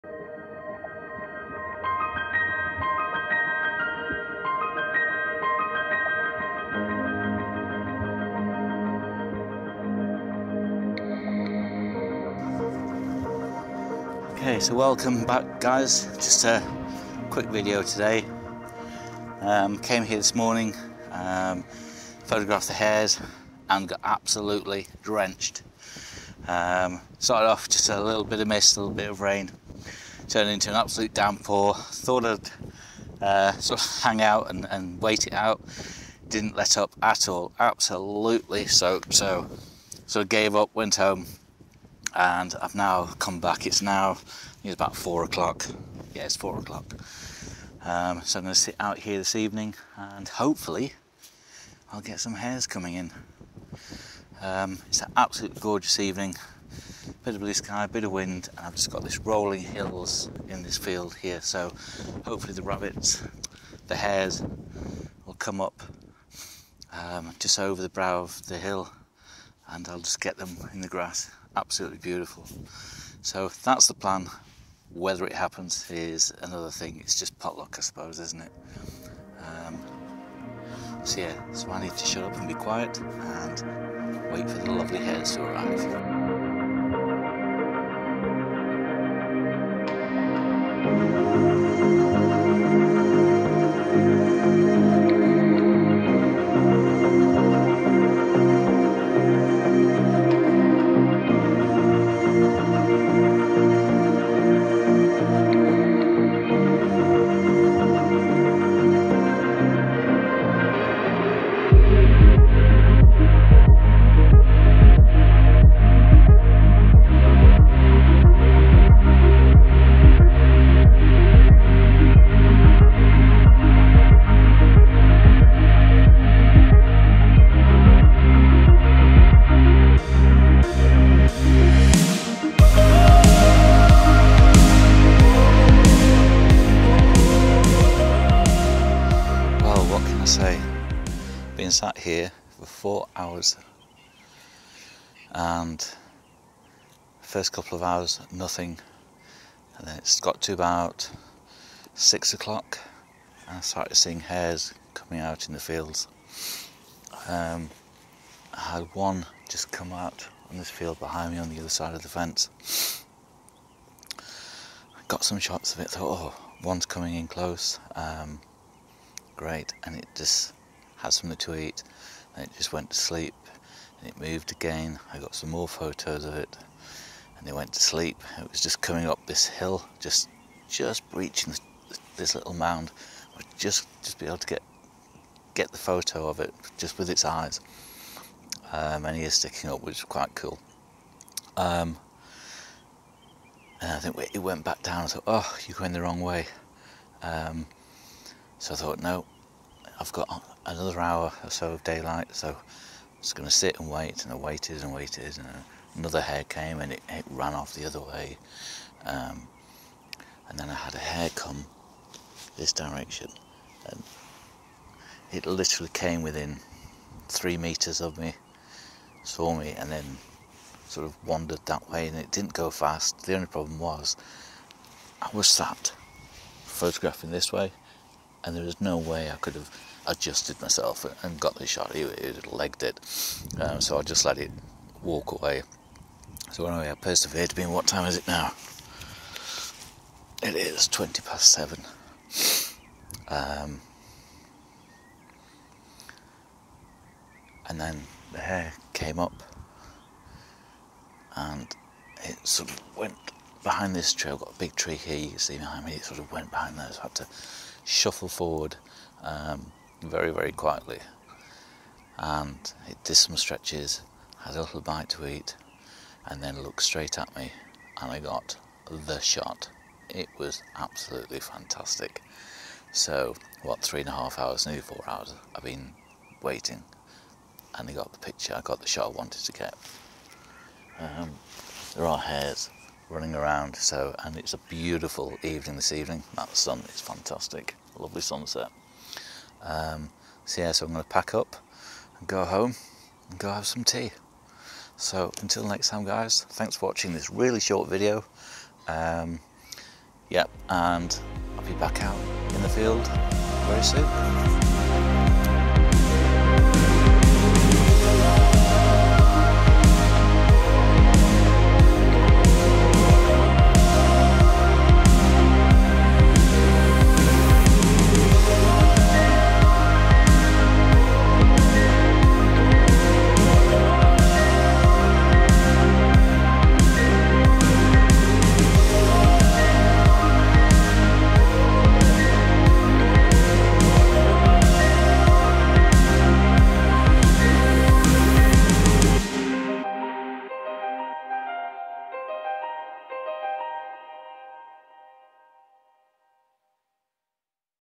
I am finished with Hares - for 2023 - photographing brown hares
So I decided to head back out to the countryside and back to the field where I photographed my last best photos of the brown hares, to try and better my images, and also just to enjoy the countryside and the pleasures of photographing wildlife.

The morning was wet wild and windy so I headed home, soon to return back in the afternoon, persevering and waiting until sunset,  my patience paid off and I managed to get the shot I wanted.

What a fantastic experience photographing these amazing and lovely animals for the first time, in my first year of wildlife photography, I am so looking forward to what the rest of the year will bring.

PHOTOGRAPHIC EQUIPMENT USED

Canon R5
Canon R6
Canon rf 100-500mm f/4.5-7.1 L
Canon rf 1.4 x convertor
Smallrig Camara cage
Benro Tortoise 24CLV Tripod

Other items used, and in my Kit

Benro Gh5c Gimbal Head
Self Adhesive Camouflage
Rain Poncho
Camera Battery Case
Tripod Stool
Camouflage scrim